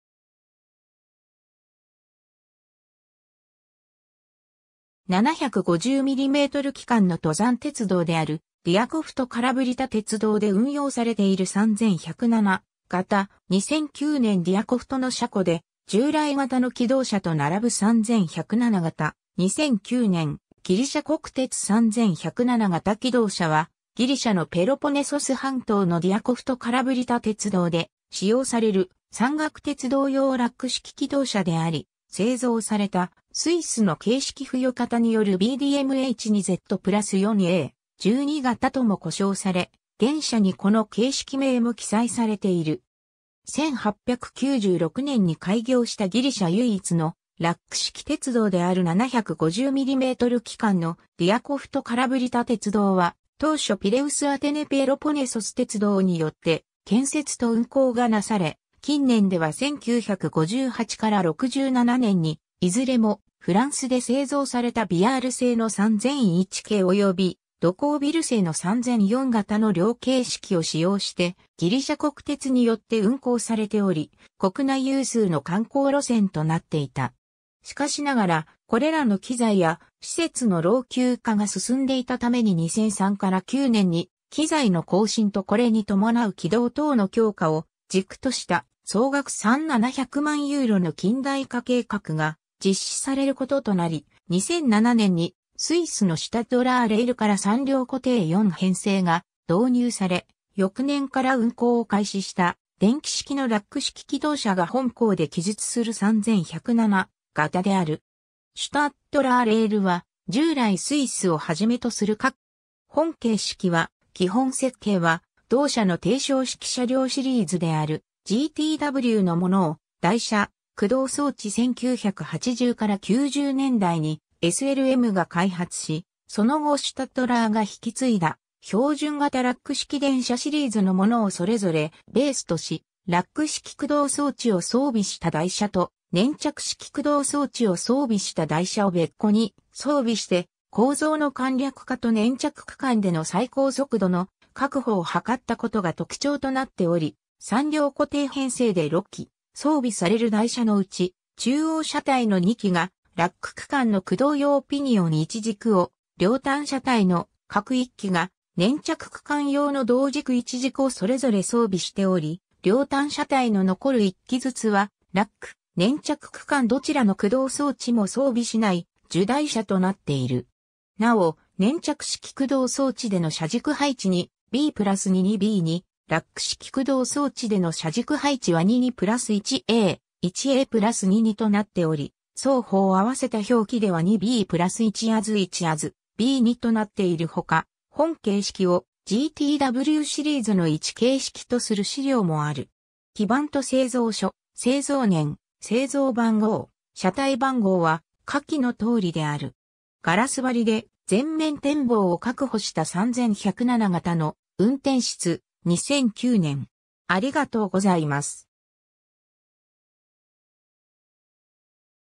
ギリシャ国鉄3107形気動車
750mm軌間の登山鉄道であるディアコフト・カラヴリタ鉄道で運用されている3107形、2009年 ディアコフトの車庫で従来型の気動車（左）と並ぶ3107形（右）、2009年 ギリシャ国鉄3107形気動車(ギリシャこくてつ3107がたきどうしゃ)は、ギリシャのペロポネソス半島のディアコフト・カラヴリタ鉄道（el:Οδοντωτός σιδηρόδρομος Διακοπτού - Καλαβρύτων）で使用される山岳鉄道用ラック式気動車であり、製造されたスイスの形式付与方によるBDmh2Z+4A/12形とも呼称され、現車にこの形式名も記載されている。
 1896年に開業したギリシャ唯一のラック式鉄道である750mm軌間のディアコフト・カラヴリタ鉄道（通称オドンドトス）は、当初ピレウス・アテネ・ペロポネソス鉄道によって建設と運行がなされ、近年では1958-67年にいずれもフランスで製造されたBillard製の3001形およびドコービル製の3004形の両形式を使用してギリシャ国鉄によって運行されており、国内有数の観光路線となっていた。
しかしながら、これらの機材や施設の老朽化が進んでいたために2003-09年に機材の更新とこれに伴う軌道等の強化を軸とした総額37百万ユーロの近代化計画が実施されることとなり、2007年にスイスのシュタッドラー・レールから3両固定4編成が導入され、翌年から運行を開始した電気式のラック式気動車が本項で記述する3107形である。
シュタッドラー・レールは、従来スイスを初めとする各国のラック式鉄道車両を手がけてきたSLMが企業再編によってSulzer-Winproとなった1998年に、同社のラック式鉄道車両部門を引継いで、その後もスイスおよびヨーロッパ向けを中心に多くのラック式鉄道車両を製造しているメーカーとなっている。
 本形式は、基本設計は同社の低床式車両シリーズであるGTWのものを、台車、駆動装置1980-90年代にSLMが開発し、その後シュタッドラーが引き継いだ標準型ラック式電車シリーズのものをそれぞれベースとし、ラック式駆動装置を装備した台車と、粘着式駆動装置を装備した台車を別個に装備して構造の簡略化と粘着区間での最高速度の確保を図ったことが特徴となっており、3両固定編成で6基装備される台車のうち、中央車体の2基がラック区間の駆動用ピニオン1軸を、両端車体の各1基が粘着区間用の動軸1軸をそれぞれ装備しており、両端車体の残る1基ずつはラック/粘着区間どちらの駆動装置も装備しない従台車となっている。
なお、粘着式駆動装置での車軸配置2‘B’+2’2’+B’2’、ラック式駆動装置での車軸配置は2’2’+1A’1A’+2’2’となっており、双方合わせた表記では2’B’+1Az’1Az'+B’2’となっているほか、本形式をGTWシリーズの一形式とする資料もある。
機番と製造所、製造年、製造番号、車体番号は下記のとおりである。
 ガラス張りで全面展望を確保した3107形の運転室、2009年